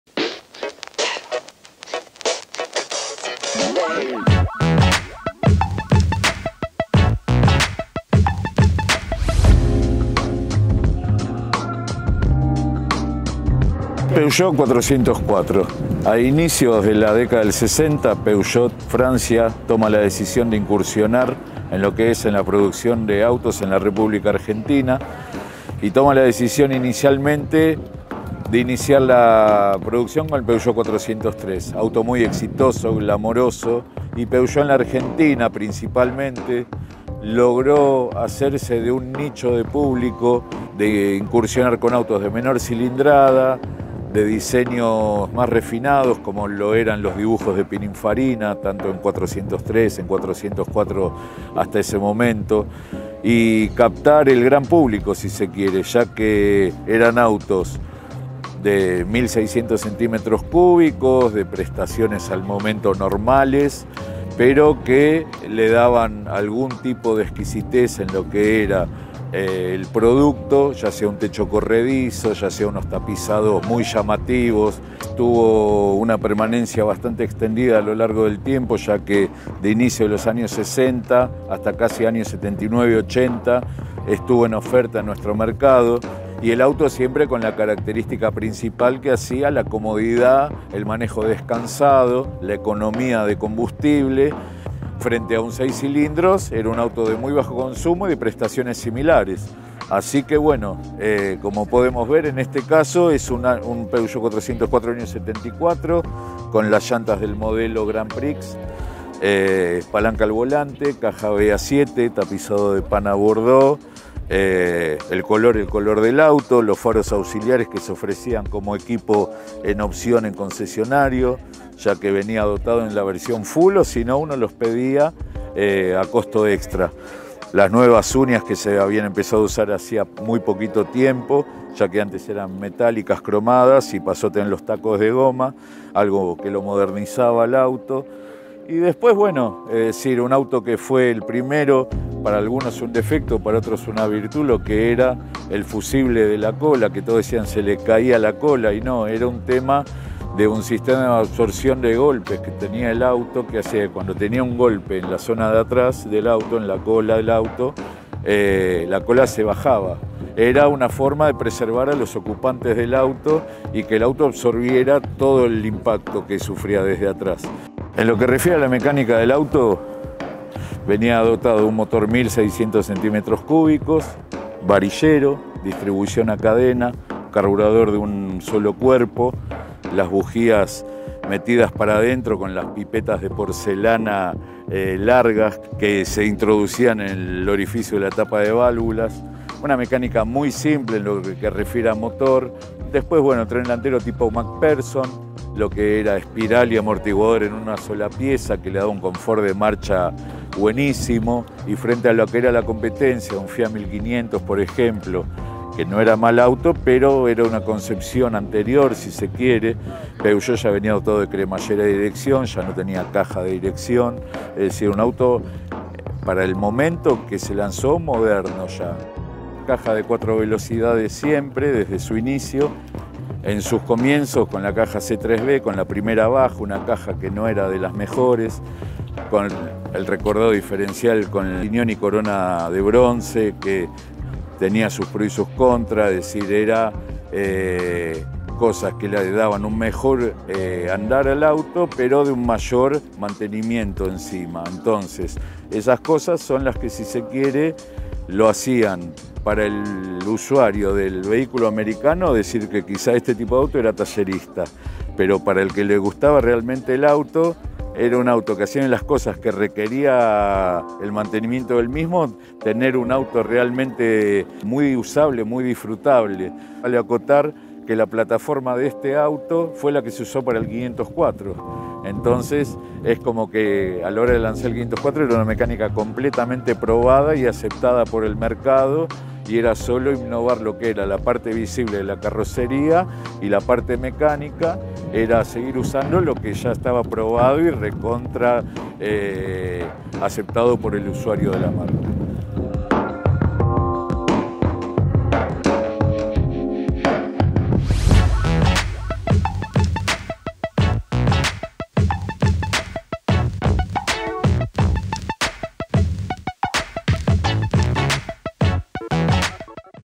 Peugeot 404 Año 1974 con kit Grand Prix - Expo Auto La Matanza | Oldtimer Video Car Garage
Oldtimer en el marco de Expo Auto en La Matanza en las mismísimas instalaciones de la ex-fábrica de Chrysler Fevre Argentina. En esta oportunidad te mostramos un hermoso Peugeot 404 de 1974 con varios accesorios de Grand Prix, como llantas, volante y luces auxiliares que le quedan muy bien. Mirá el video para más detalles. Dejanos tu opinión abajo.